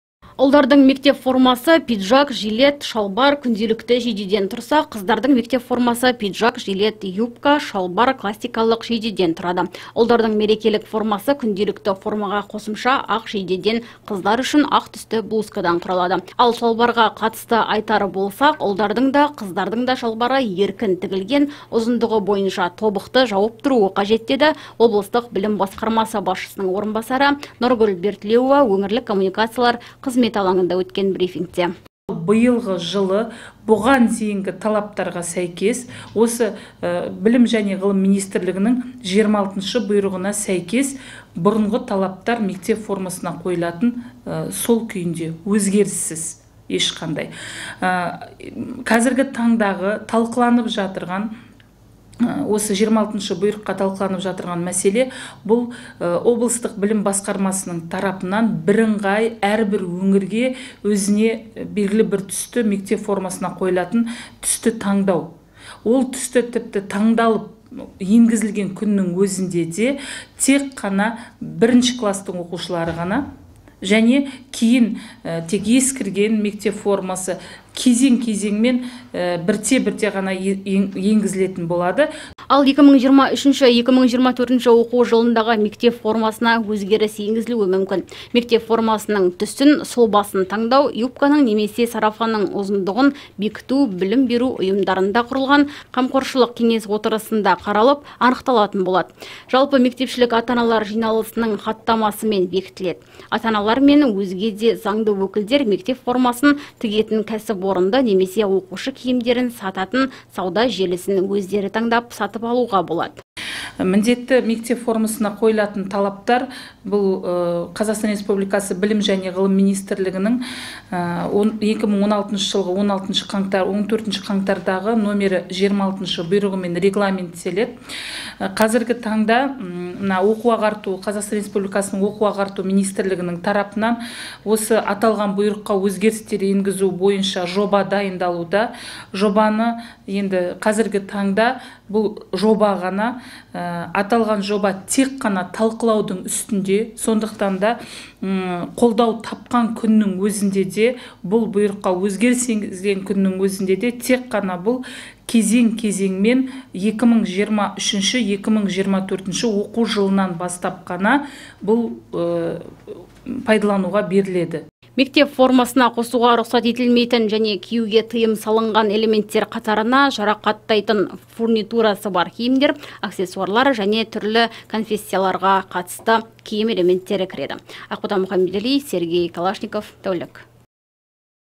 Келесі оқу жылында мектеп формасы өзгермейді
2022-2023 оқу жылында мектеп формасы өзгермейді. Форманың сұлбасы, түсі классикалық стильде, бірыңғай түс гаммасында жасалады, үшеуден асырмай түстерді араластыруға рұқсат етілген. Мектеп формасы білім алушылардың жас ерекшеліктеріне қарай енгізіледі. Ол күнделікті, мерекелік және спорттық болып бөлінеді.Ұлдардың мектеп формасы пиджак, жилет, шалбар, күнделікті жейдеден тұрса, қыздардың мектеп формасы пиджак, жилет, юбка, шалбар, классикалық жейдеден тұрады. Ұлдардың мерекелік формасы күнделікті формаға қосымша ақ жейдеден, қыздар үшін ақ түсті блузкадан құралады. Ал шалбарға қатысты айтар болсақ, ұлдардың да, қыздардың да шалбары еркін тігілген, ұзындығы бойынша тобықты жауып тұруы қажет, деді облыстық білім басқармасы басшысының орынбасары Нұргүл Бертілеуова өңірлік коммуникациялар қызметі алаңында өткен брифингте. Ал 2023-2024 оқу жылындағы мектеп формасына өзгеріс енгізілуі мүмкін. Мектеп формасының түсін, сұлбасын таңдау, юбканың немесе сарафанның ұзындығын бекіту білім беру ұйымдарында құрылған қамқоршылық кеңес отырыстарында қаралып, анықталатын болады. Жалпымектепішілік ата-аналар жиналысының хаттамасымен бекітіледі. Ата-аналар мен өзге де заңды өкілдер мектеп формасын тігетін кәсіпорынды немесе оқушы киімдерін сататын сауда желісін өздері таңдап, сатып алуына болады. Мектеп формасына қосуға рұқсат етілмейтін және киюге тыйым салынған элементтер қатарына жарақаттайтын фурнитурасы бар киімдер, аксессуарлар және түрлі конфессияларға қатысты киім элементтері кіреді.